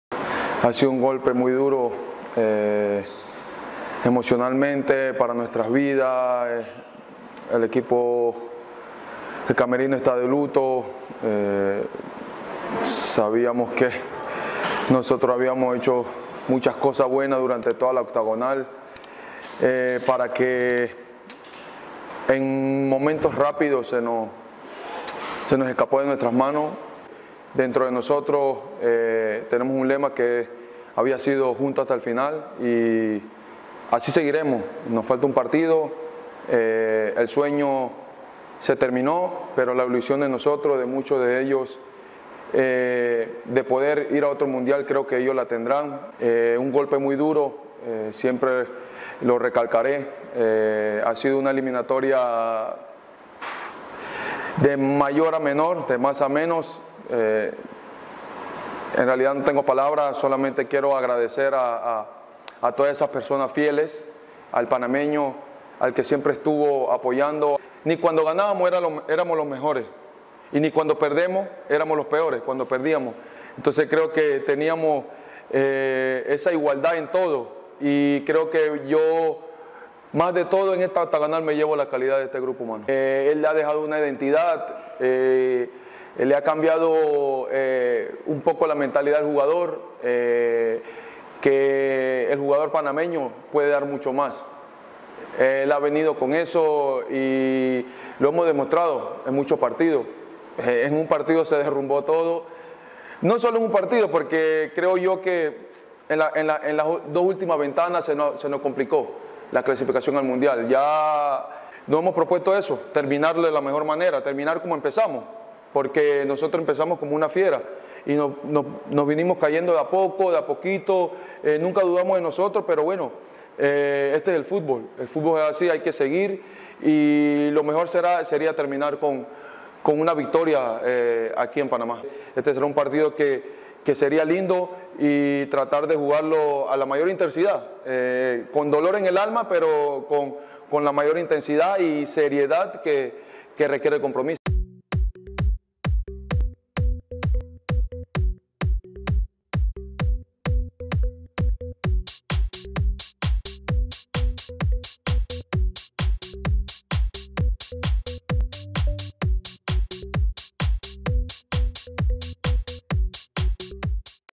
La TRISTEZA de Aníbal Godoy por la ELIMINACIÓN de Panamá de Qatar 2022
Entrevista con Aníbal Godoy, previo al último partido del Octagonal para la Selección Panameña tras su eliminación.

🍀 Regístrate en 1xBet con este link y llévate un bono de hasta 130 euros en tu primer depósito